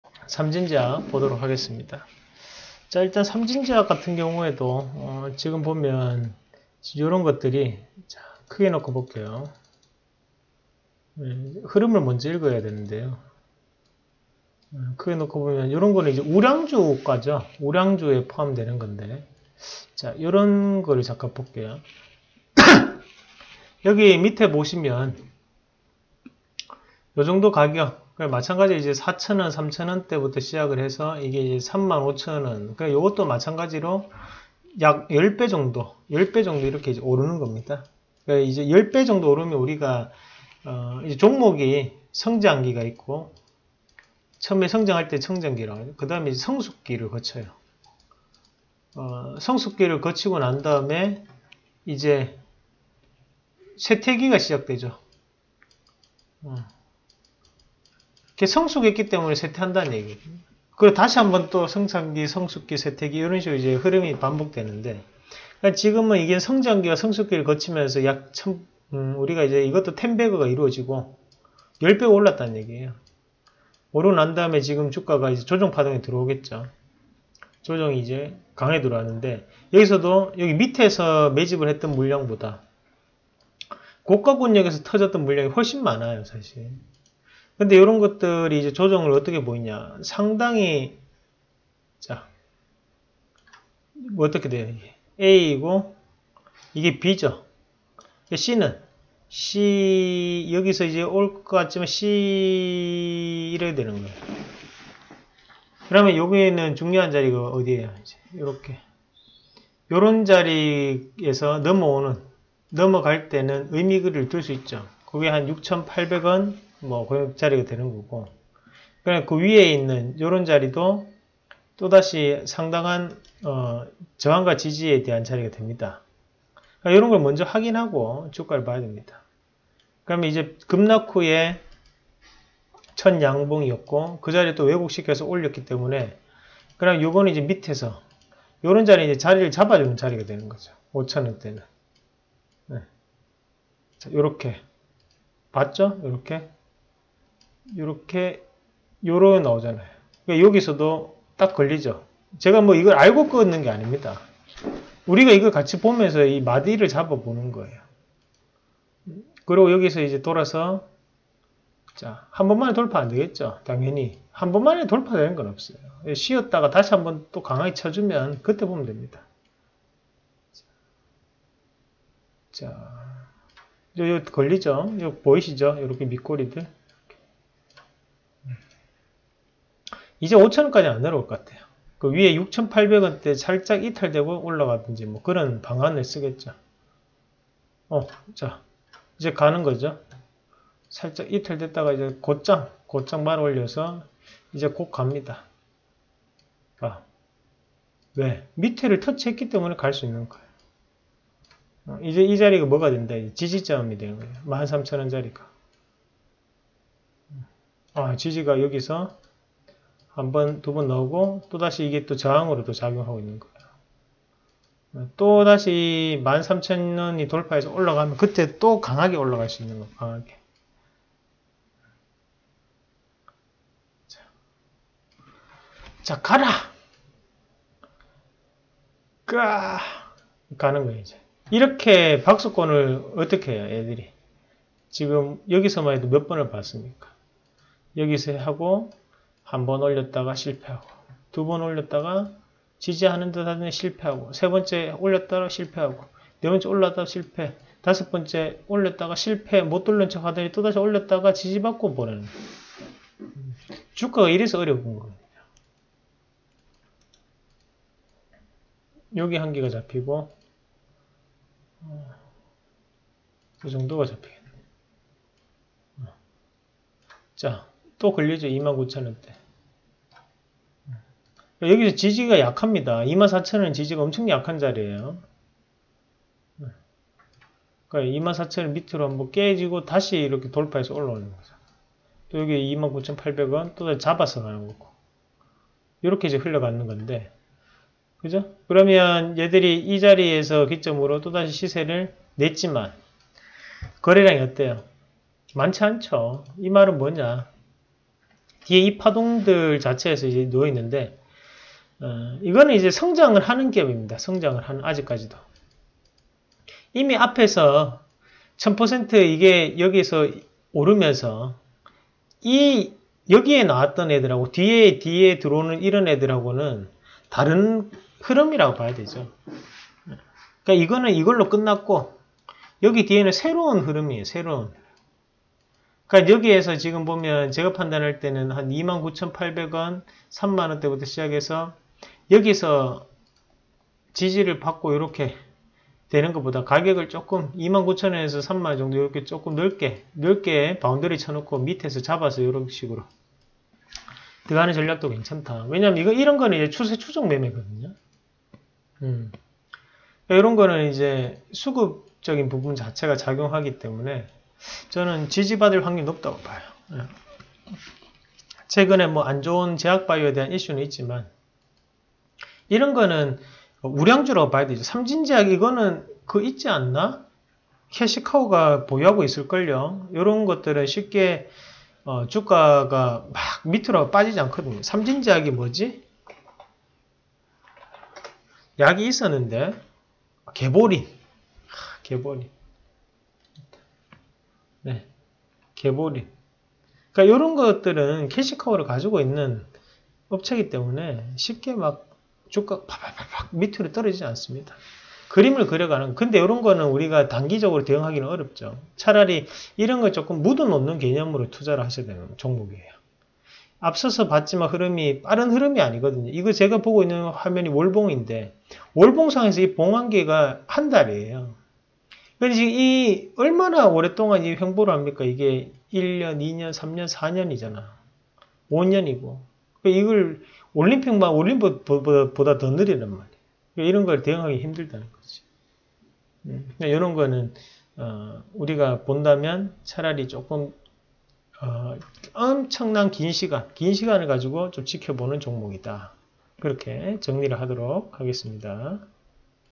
삼진제약 우량주? 기관,외국인 매수하는 종목 분석하는 방법, 추세구분하는 방법, 세력매집가격, 게보린 이상로의 차트분석
1. 삼진제약은 우량주다?
2. 기관,외국인 매수하는 종목 분석방법
3. 삼진제약의 추세구분하기